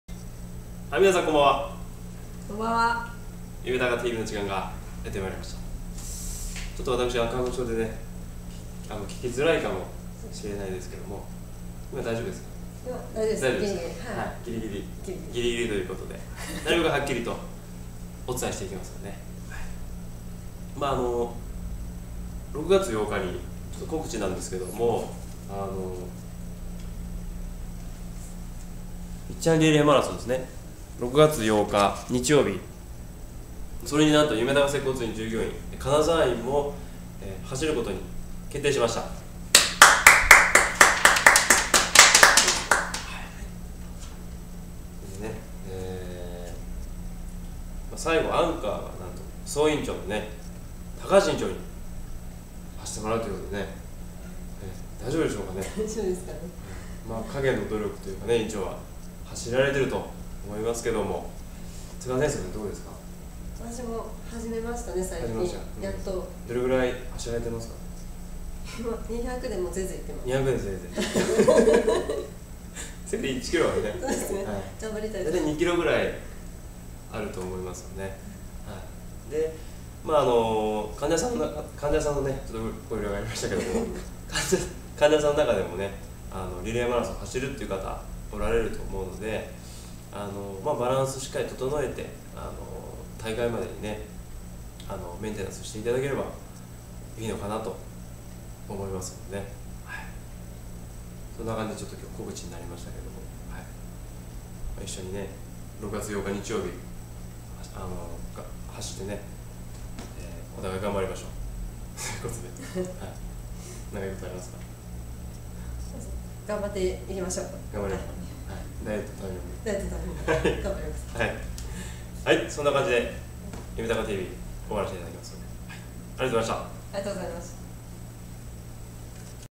本日はちょっとした告知です！
６月に行われる、いっちゃんリレーマラソンにゆめたか接骨院
参加します！
アンカーは総院長の高橋院長を予定しています(*'▽')